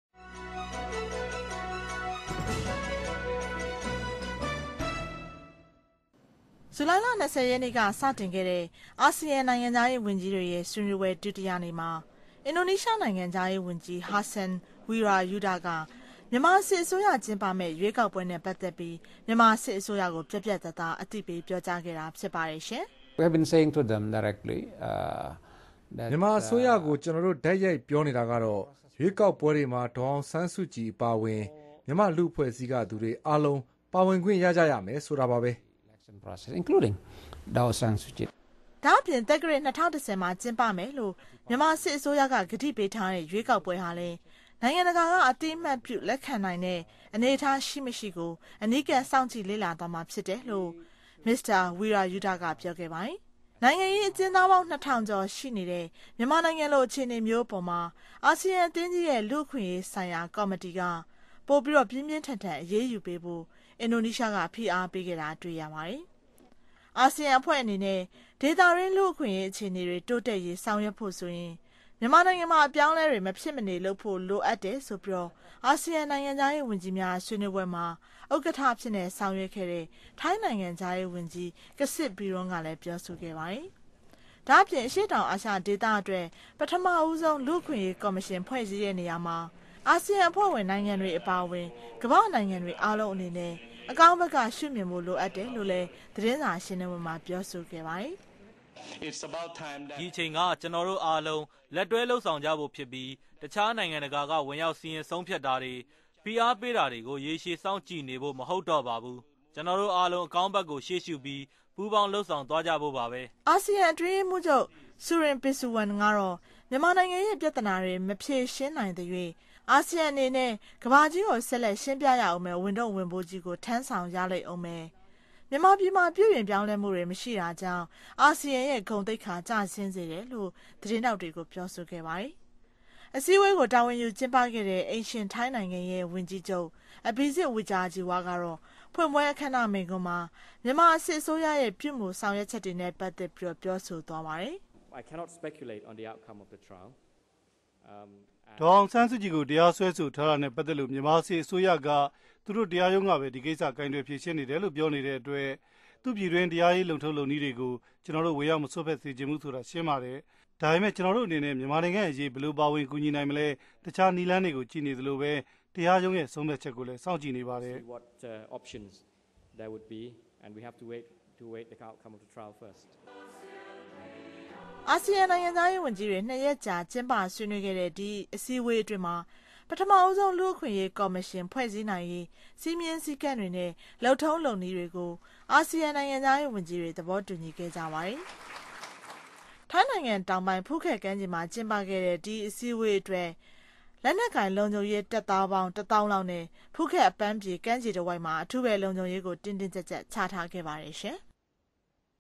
ASEAN Leaders Urge Military Regime for Inclusive Elections (VOA Burmese)
Indonesia Foreign Minister demands the military regime to release pro-democracy Aung San Suu Kyi in order to receiving trust of international community. The 42nd Association of Southeast Asian Nations (ASEAN) Ministerial Meeting and the 16th ASEAN Regional Forum will be held in Phuket, Thailand on July 20-23, 2009.